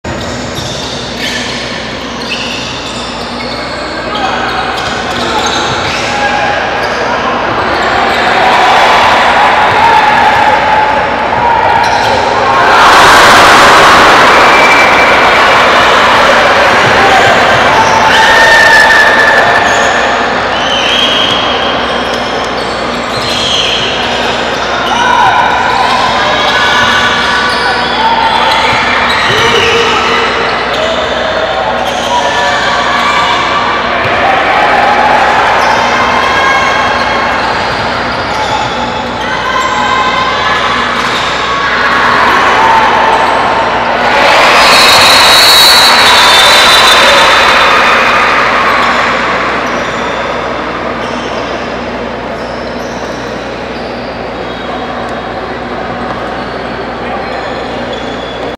Academia venció a Sabios de Caldas Liga Directv Baloncesto Colombiano
cademia Basket Club de Antioquia venció a Sabios de Caldas, la gran figura para los antioqueños fue Jonathan Rodríguez de Puerto Rico junto al norteamericano Murray Tyler , el quinteto de Hernán Darío Giraldo contó además con la presencia de jugadores como Juan Fajardo, Simon Granados, Darry Smith, Mauricio Montaño entre otros, el cubrimiento fue realizado por Jorge Mario Zapata Jaramillo, David Julián Ramírez, D´Mentes Productivas para colombiasports.net